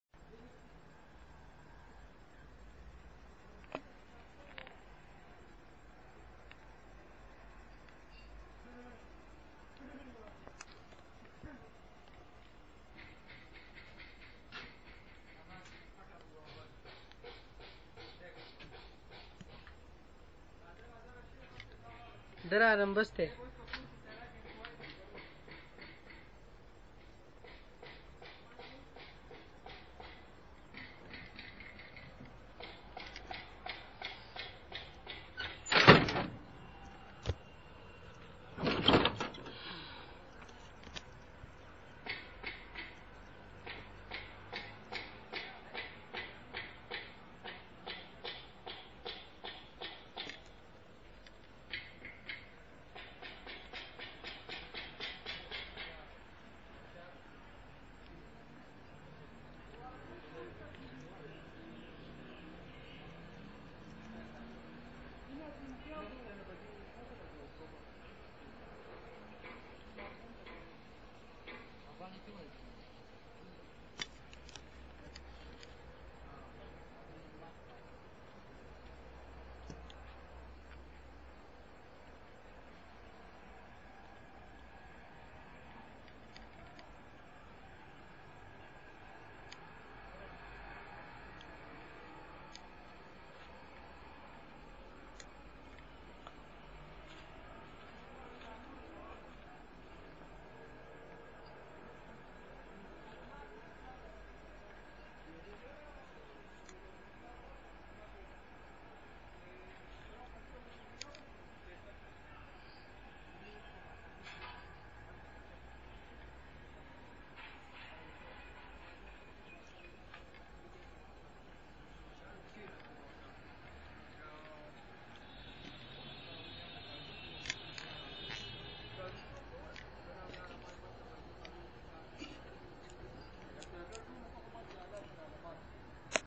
Oldest bus in Govt PG Jahanzeb college جھانزیب کالج میں پرانا بس
this oldest bus of jahanzeb college stand there i think from 50 years and beacause of no use of it it becomes idle.
Jahanzeb college saidu sharif swat